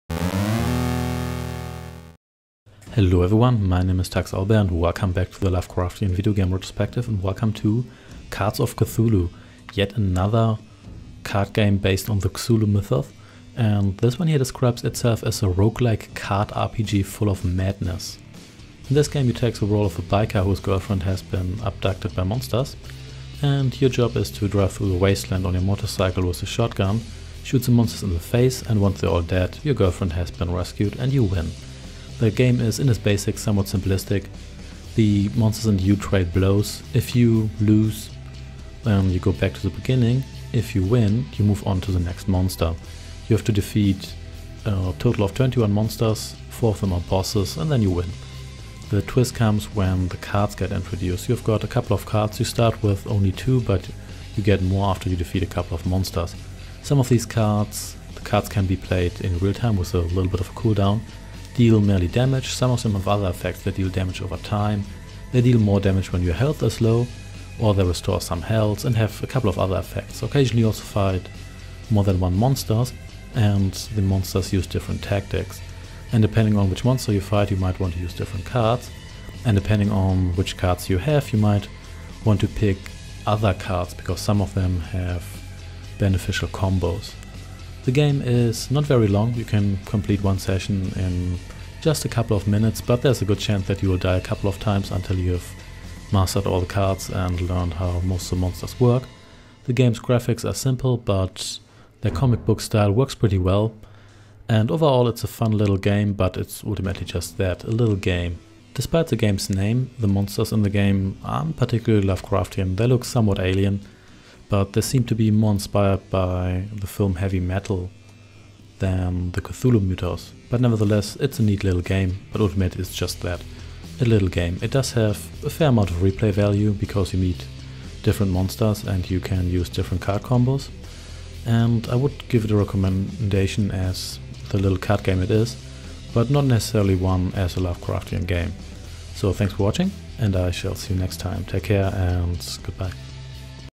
Cards of Cthulhu | Lovecraftian Game Retrospective
Here's another card game based on the Cthulhu mythos!

Welcome to the Lovecraftian Video Game Retrospective. Here, we will have a look at a number of video games inspired or based on the works of H.P. Lovecraft.